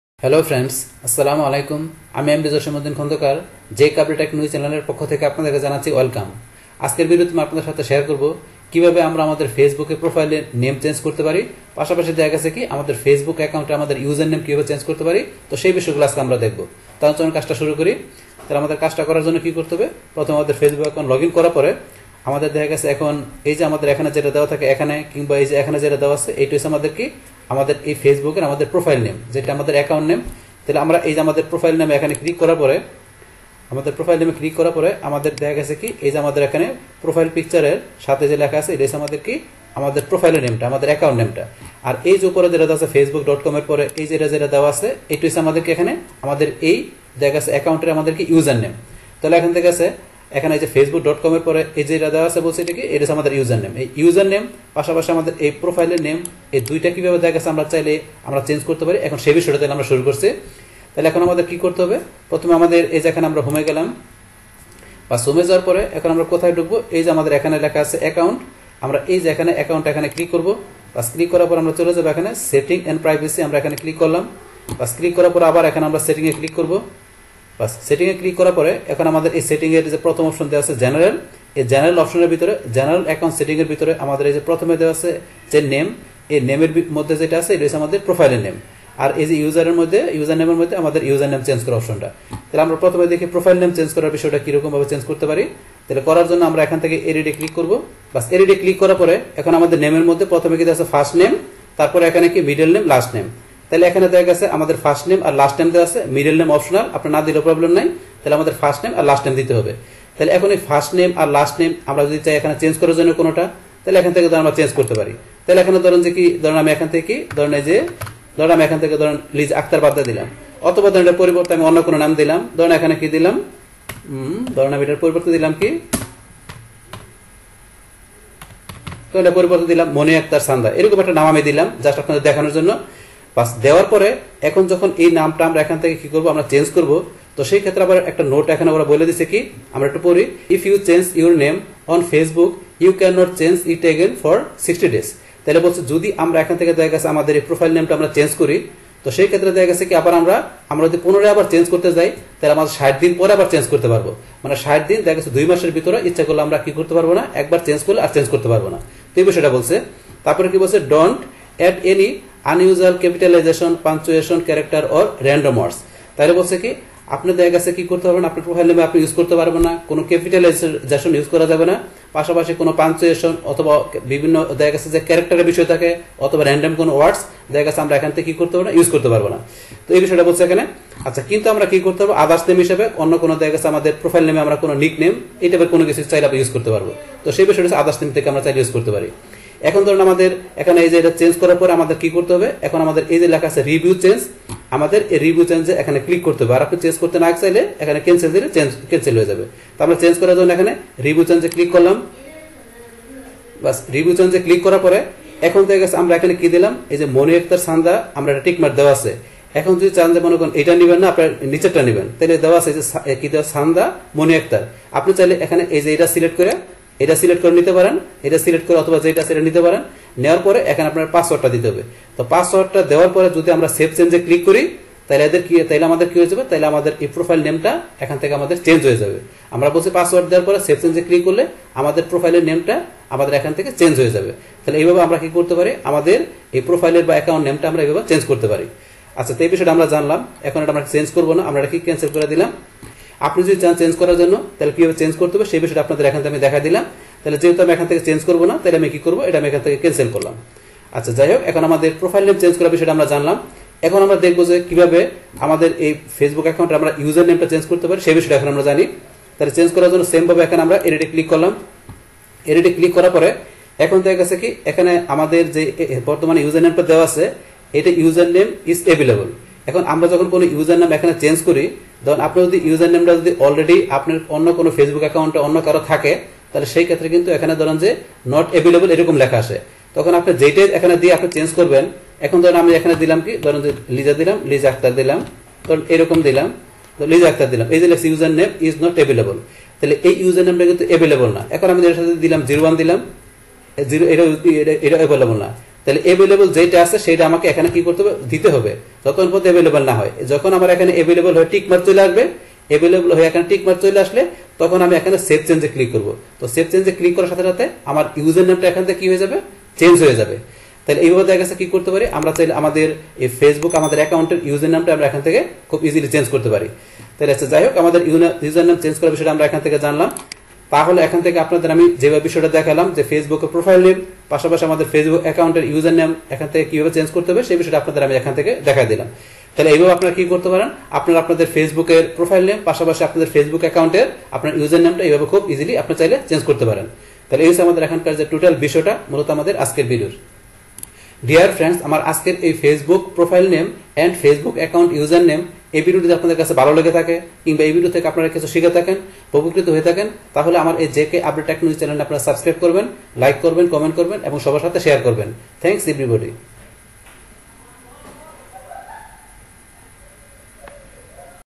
How To Change Advanced Facebook Profile Name & Username - Bangla
In this video's I will show How To Change Advanced Facebook Profile Name & Username Bangla Tutorial.